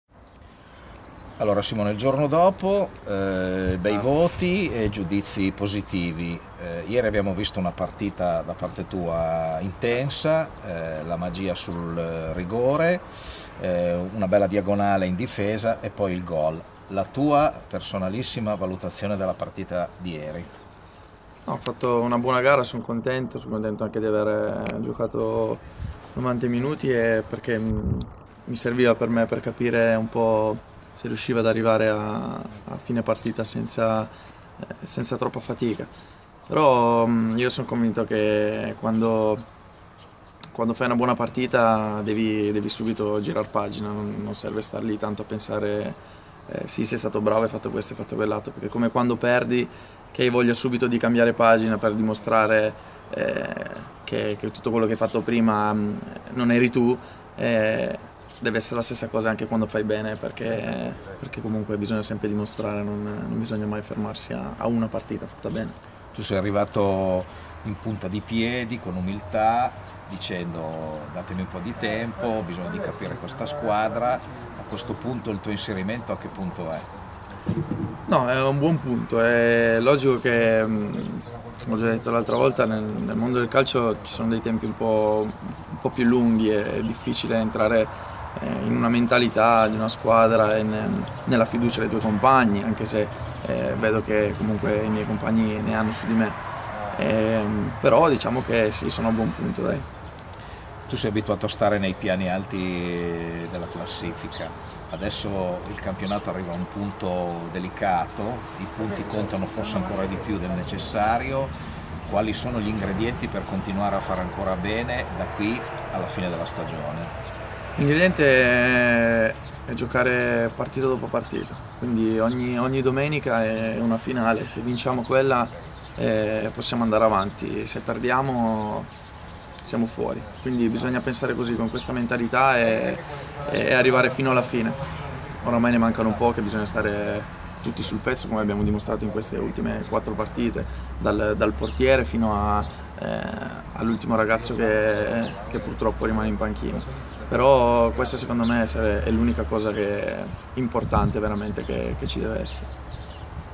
La videointervista...Simone Iocolano
Giana-Alessandria 0-3: il giorno dopo con Simone Iocolano, a cura dell'Ufficio Stampa